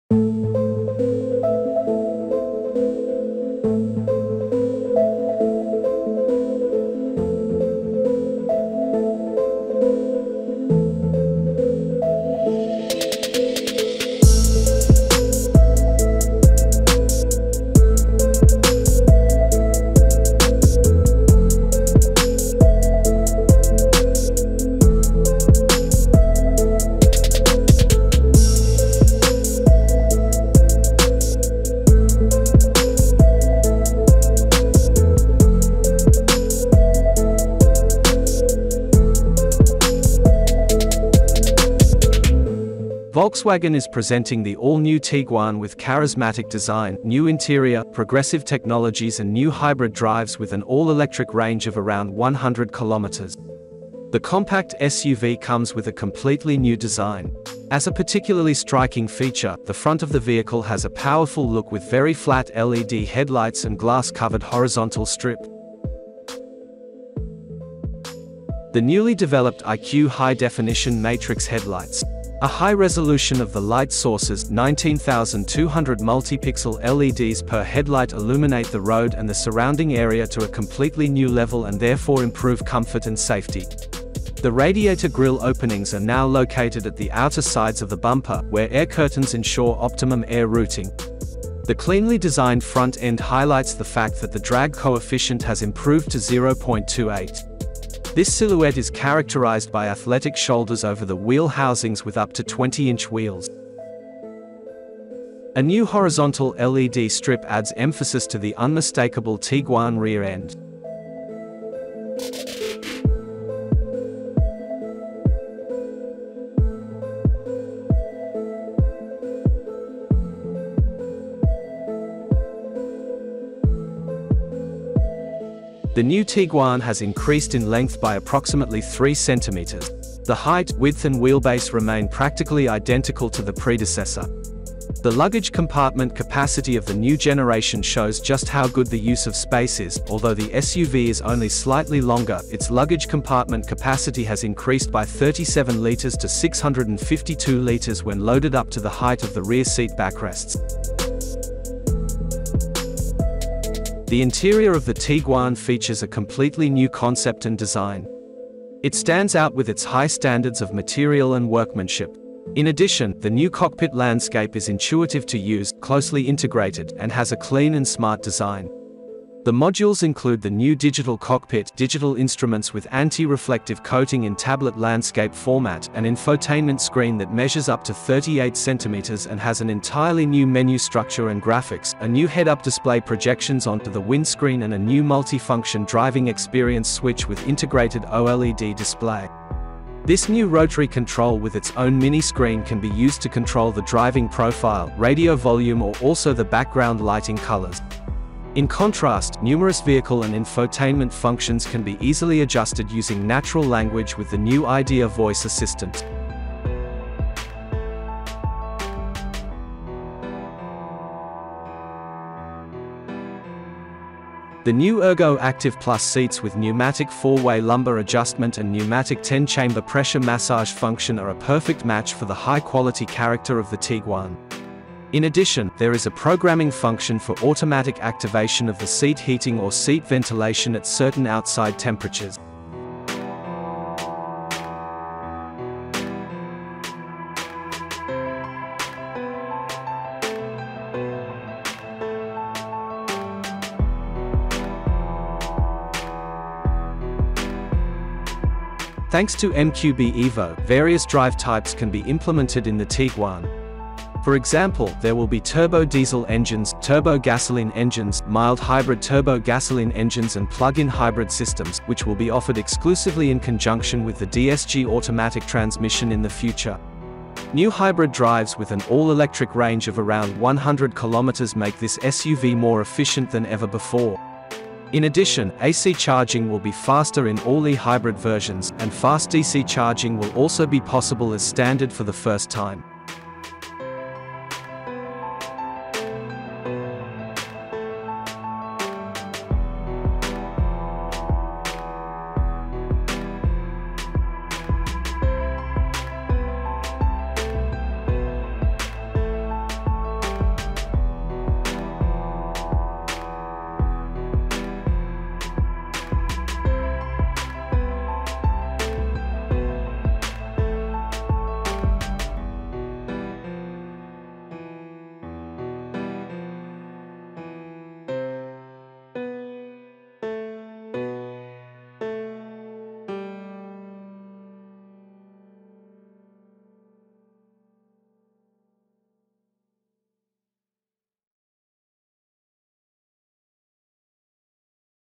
2024 Volkswagen Tiguan vs Old Volkswagen Tiguan
2024 Volkswagen Tiguan R-Line vs Old Volkswagen Tiguan R-Line
Comparison Visual Exterior & Interior Design

Volkswagen is presenting the all-new Tiguan with charismatic design, new interior, progressive technologies and new hybrid drives with an all-electric range of around 100 km.

The compact SUV comes with a completely new design. As a particularly striking feature, the front of the vehicle has a powerful look with very flat LED headlights and glass-covered horizontal strip.

The Tiguan to be launched with the newly developed IQ.LIGHT HD (High Definition matrix) headlights. A high resolution of the light sources, 19,200 multipixel LEDs per headlight illuminate the road and the surrounding area to a completely new level and therefore improve comfort and safety.

The radiator grille openings are now located at the outer sides of the bumper, where air curtains ensure optimum air routing. The cleanly designed front end highlights the fact that the drag coefficient has improved from 0.33 to 0.28. This silhouette is characterised by athletic shoulders over the wheel housings (with up to 20-inch wheels). A new horizontal LED strip adds emphasis to the unmistakeable Tiguan rear end.

The new Tiguan has increased in length by approximately three centimetres. The height, width and wheelbase remain practically identical to the predecessor. The luggage compartment capacity of the new generation shows just how good the use of space is: although the SUV is only slightly longer, its luggage compartment capacity has increased by 37 litres to 652 litres (when loaded up to the height of the rear seat backrests).

The interior of the Tiguan features a completely new concept and design. It stands out with its high standards of material and workmanship. In addition, the new cockpit landscape is intuitive to use, closely integrated, and has a clean and smart design. The modules include the new Digital Cockpit (digital instruments with anti-reflective coating in tablet landscape format), an infotainment screen that measures up to 38 centimetres (15 inches) and has an entirely new menu structure and graphics, a new head-up display (projections on to the windscreen) and a new multifunction driving experience switch with integrated OLED display. This new rotary control with its own mini screen can be used to control the driving profile, radio volume or also the background lighting colours. In contrast, numerous vehicle and infotainment functions can be easily adjusted using natural language with the new IDA voice assistant.

The new ergoActive Plus seats with pneumatic four-way lumbar adjustment and pneumatic 10-chamber pressure massage function are a perfect match for the high-quality character of the Tiguan. In addition, there is a programming function for automatic activation of the seat heating or seat ventilation at certain outside temperatures.

Thanks to MQB evo, various drive types can be implemented in the Tiguan. For example, there will be turbo diesel engines (TDI), turbo gasoline engines (TSI), mild hybrid turbo gasoline engines (eTSI) and plug-in hybrid systems (eHybrid), which will be offered exclusively in conjunction with the automatic transmission (DSG) in the future.

New hybrid drives with an all-electric range of around 100 km make this SUV more efficient than ever before. In addition, AC charging will be faster in all eHybrid versions, and fast DC charging will also be possible as standard for the first time.